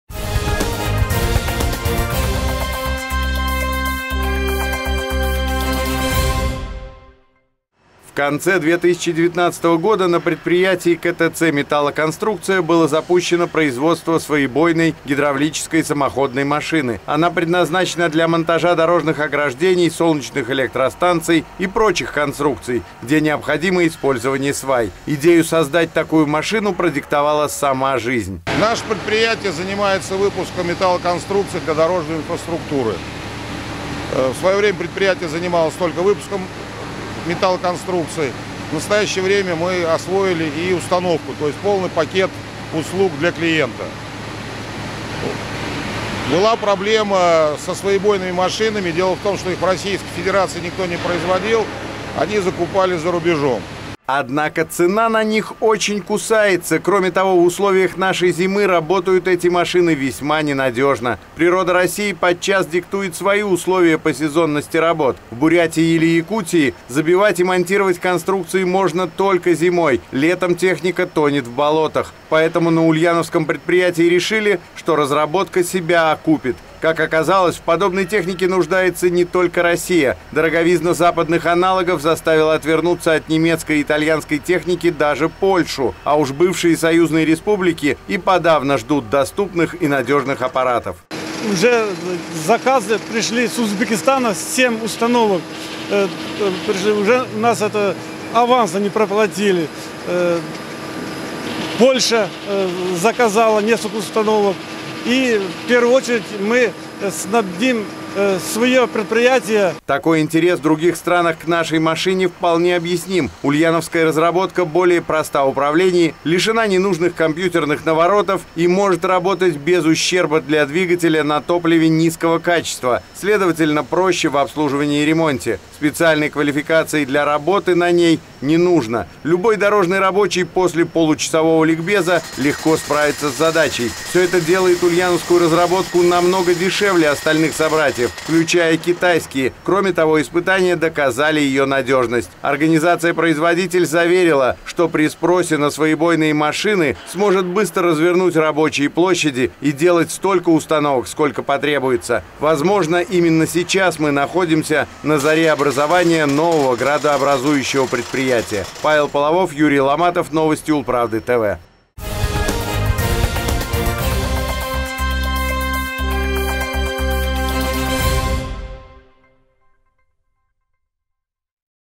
Бой-машина. Ульяновские разработчики покоряют мир уникальной установкой
Конструкторско-технологический центр «Металлоконструкция» переходит на массовое производство отечественной сваебойной машины. Это большой прорыв не только для Ульяновска, но и для мира в целом.
В конце 2019 года на предприятии КТЦ «Металлоконструкция» было запущено производство сваебойной гидравлической самоходной машины. Она предназначена для монтажа дорожных ограждений, солнечных электростанций и прочих конструкций, где необходимо использование свай. Идею создать такую машину продиктовала сама жизнь.